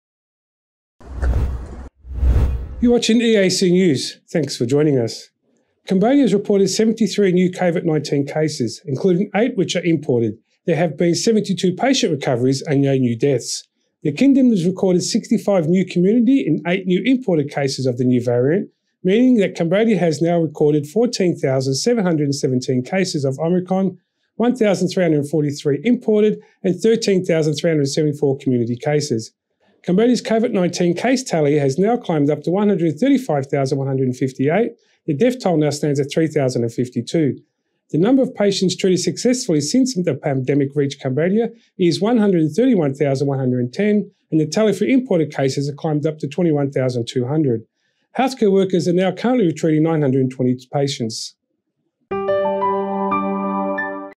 Cambodia Releases Daily Covid-19 Statistics 22 March 2022
Phnom Penh, 22 March 2022: Cambodia has reported 73 new Covid-19 cases. This includes 8 imported cases. There have been 72 patient recoveries and 0 deaths.

The information was released by the Ministry of Health just before Tuesday lunchtime.

The total of local Omicron cases have achieved 13,374 cases including 65 new cases today.

Cambodia's Covid-19 case tally has now climbed to 135,158. The death toll stands at 3,052. The number of patients treated successfully since the pandemic reached Cambodia is 131,110 and case tally imported has climbed to 21,200.